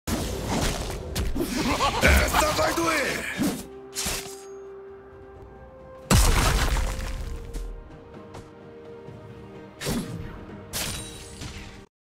Q feros VS R Show 100% da eficiência na skill usada
Eu fiquei besta quando Warwick usou Q-Pressas da Fera carregado para sair do controle do R-Hora do Show, foi realmente uma demonstração de mecânica que não se vê todo dia no Bronze IV😤 deem a este homem um troféu e que mais inimigos fortes caiam comigo 👊